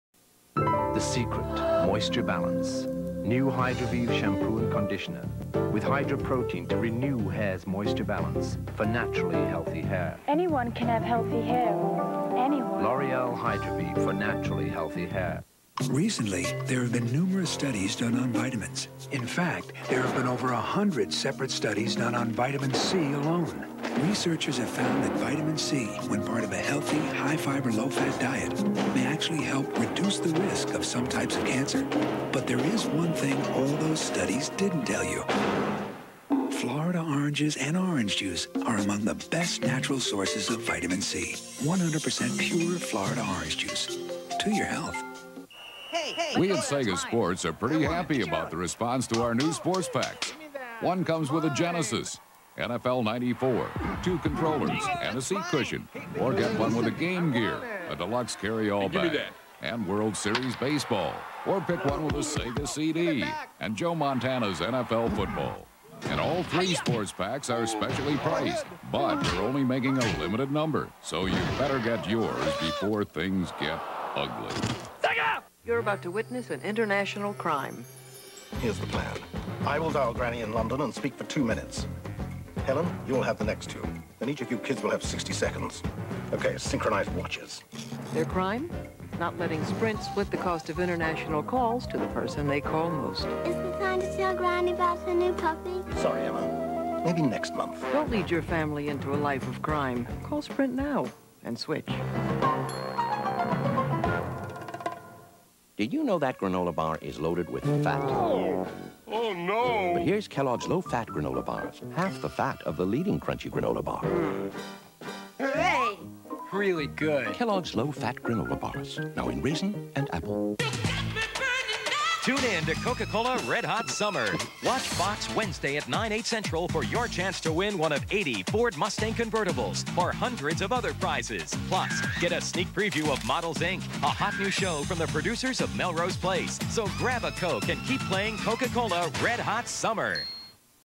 90s Commercials (1994)
Commercials on FOX (1994)

L'Oreal Hydravive
Florida Orange Juice
Sega Sports Pack
Sprint
Kellogg's Low Fat Granola Bars
Coca Cola Red Hot Summer